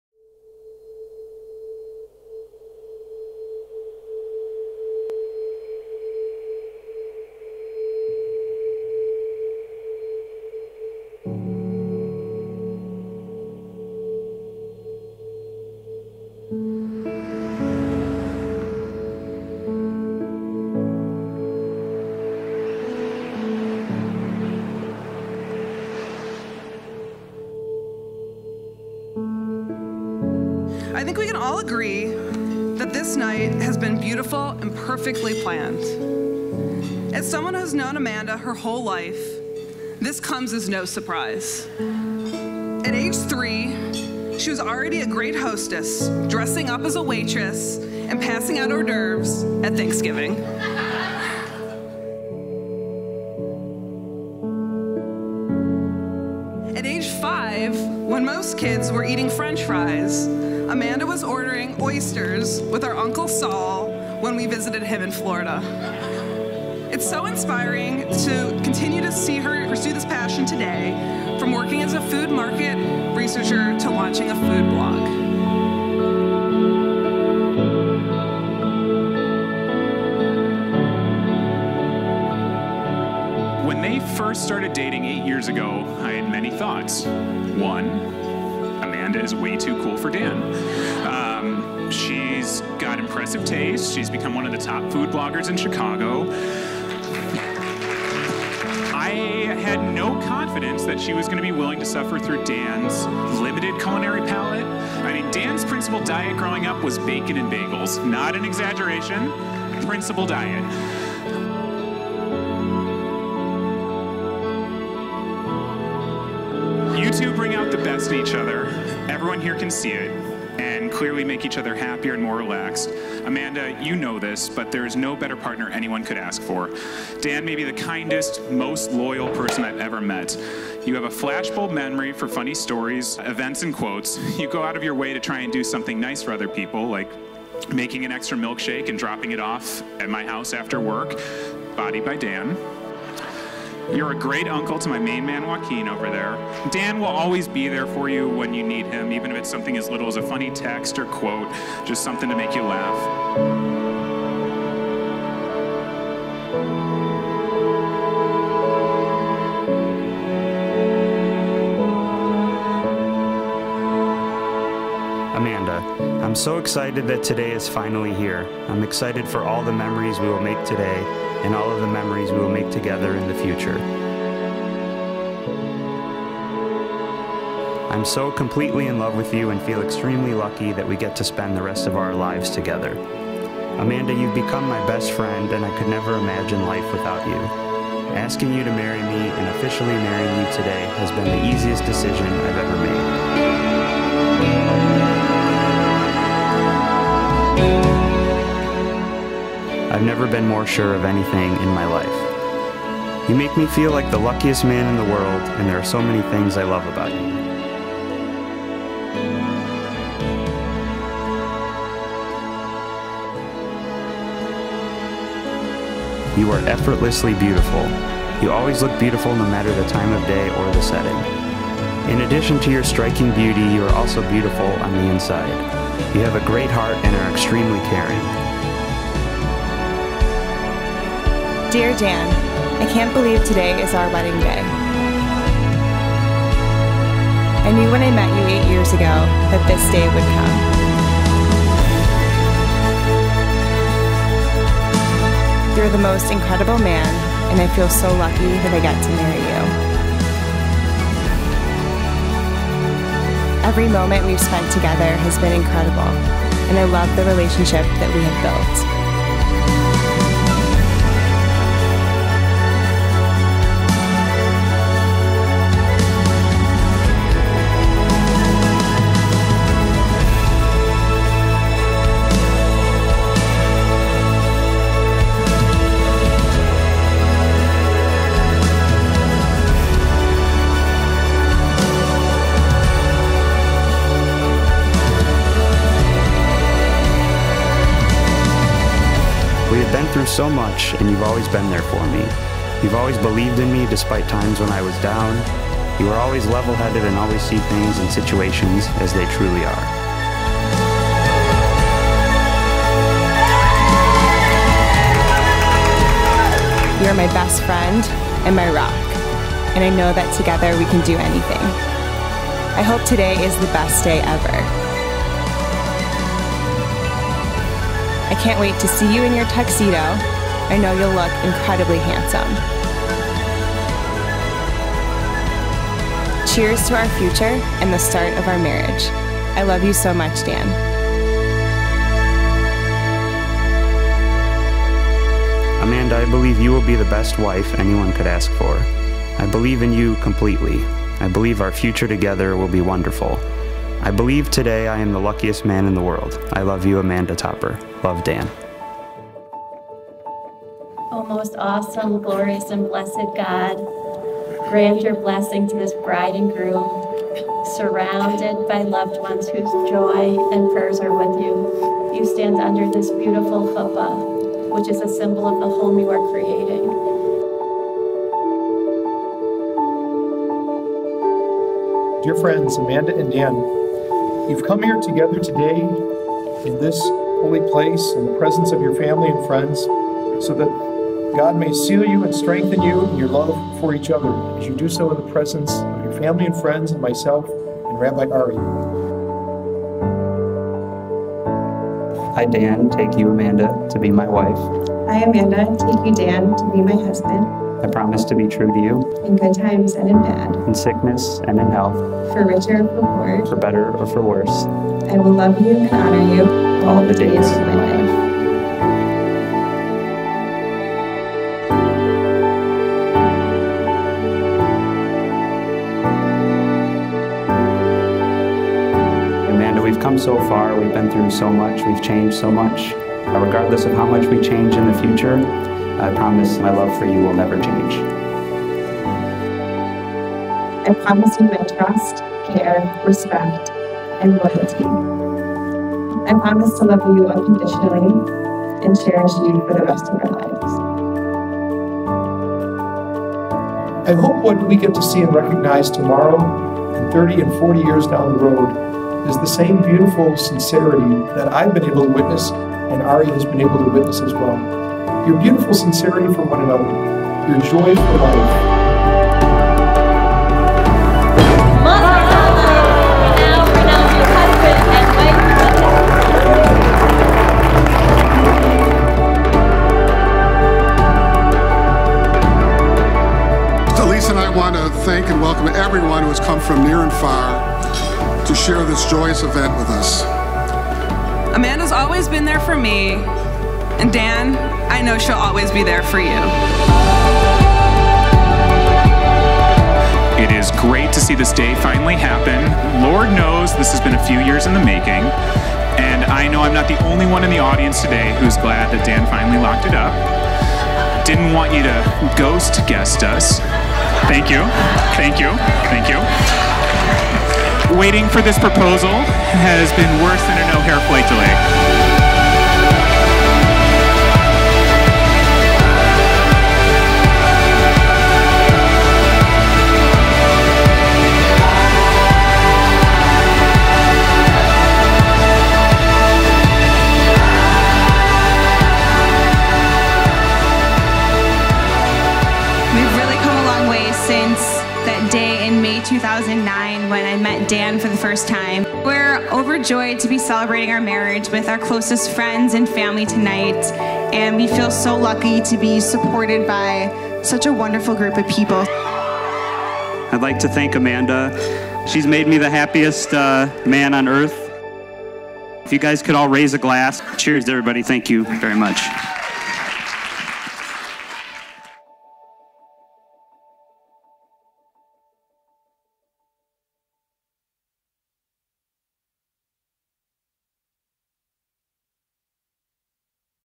Langham Chicago Wedding Video by 312FILM videography
Chicago Luxury Wedding Videography | Amanda & Dan

All aerials filmed with a licensed pilot.

THE TEAM:
Cinematography | 312FILM